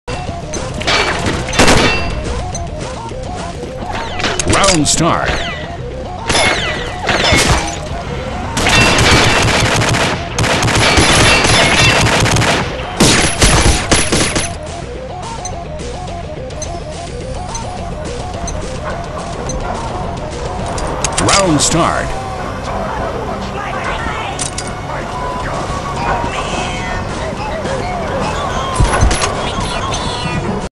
Hacker Report xM0DD3Rx
Hacker Report: OPK Yes was officially reported and is now banned. CBL still pending.